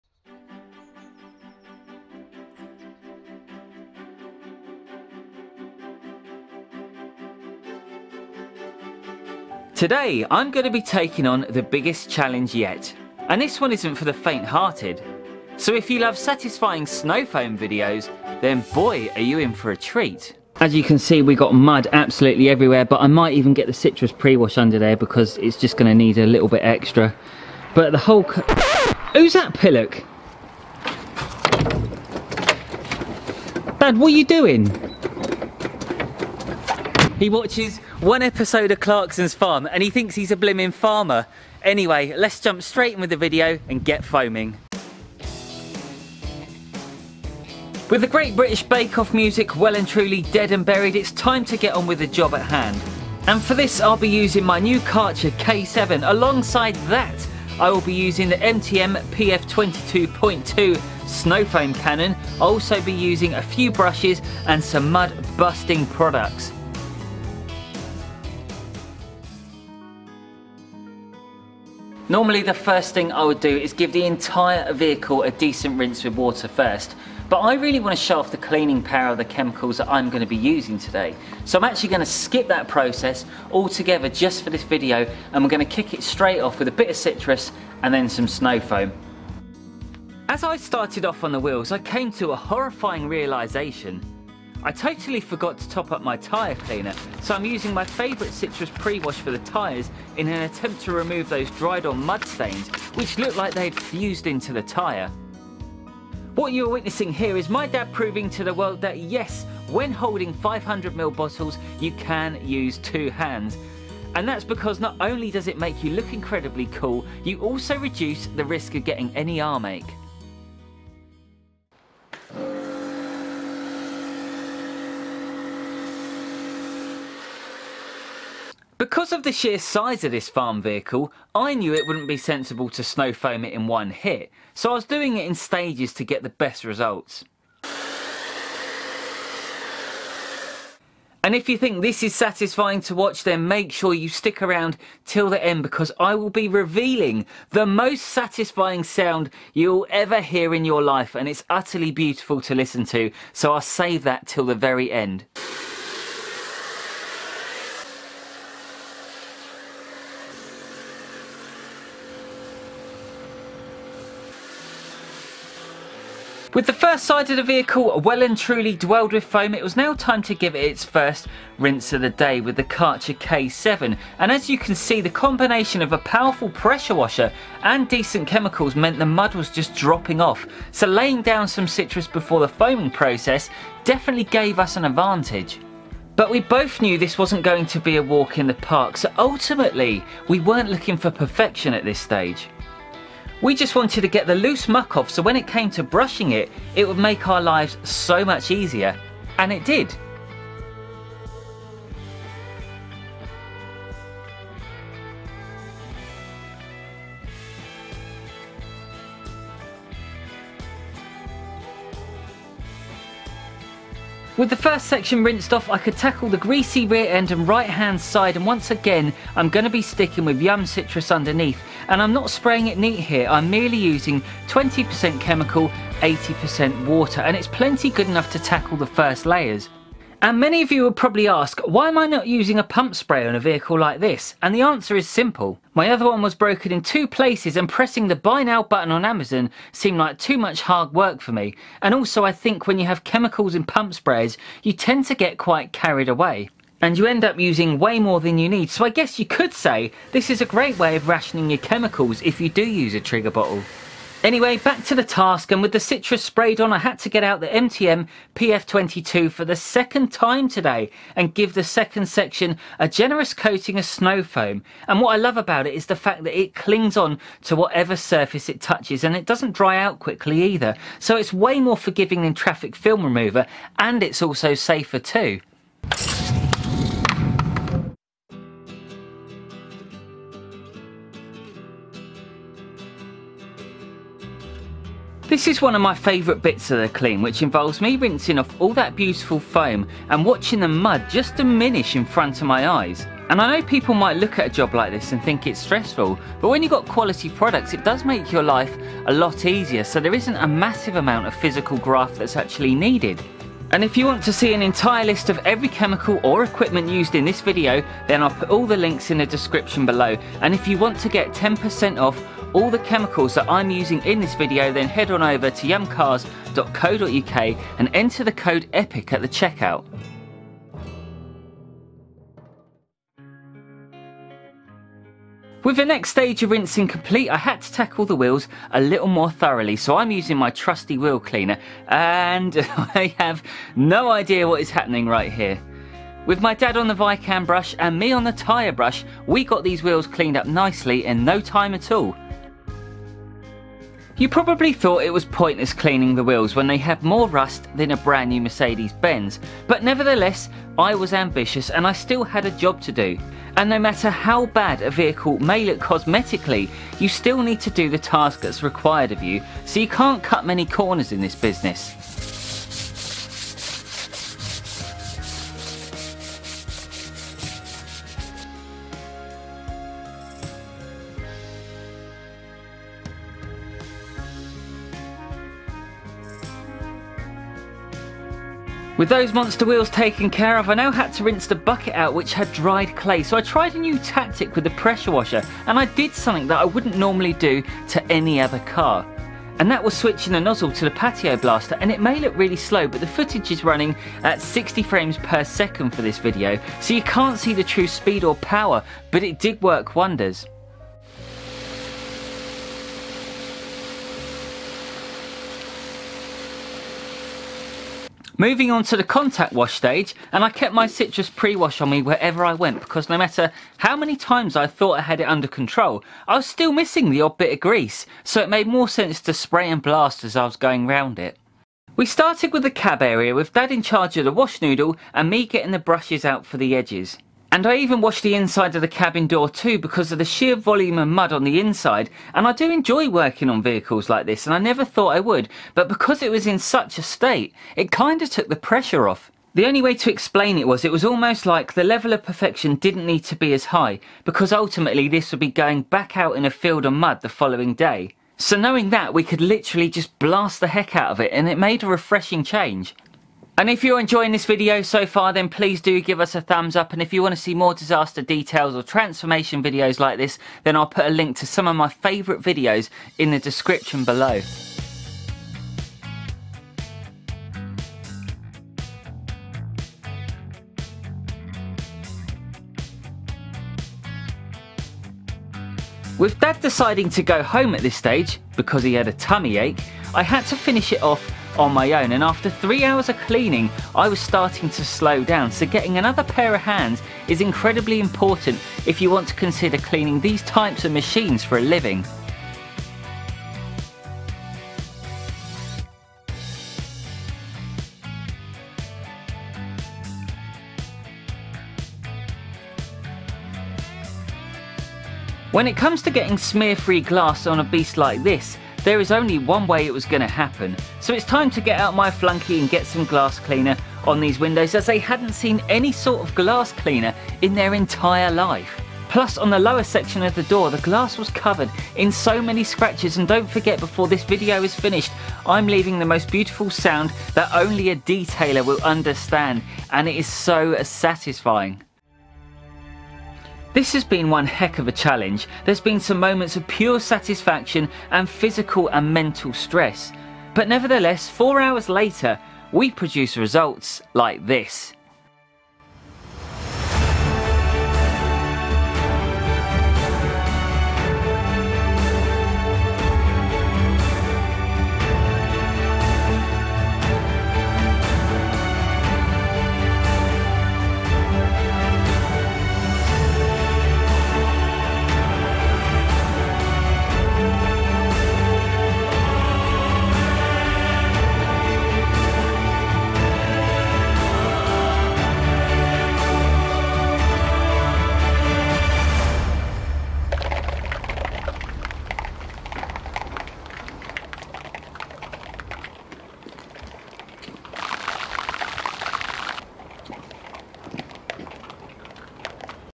FIRST WASH IN 4 YEARS!!! Pressure Washing a MUDDY TRACTOR
This tractor Received its first wash in 4 years. Deep Cleaning my biggest challenge be yet!!! Snow Foaming a Farmer's Muddy Telehandler using the Karcher K7 Smart Control Pressure Washer.

This muddy telehandler which required plenty of citrus pre wash, loads of snow foam and plenty of satisfaction guaranteed.

//Wheel Brushes & Wash Tools:

Vikan brush for tyres
G Techniq Wash Mitt
Kent Wash Noodle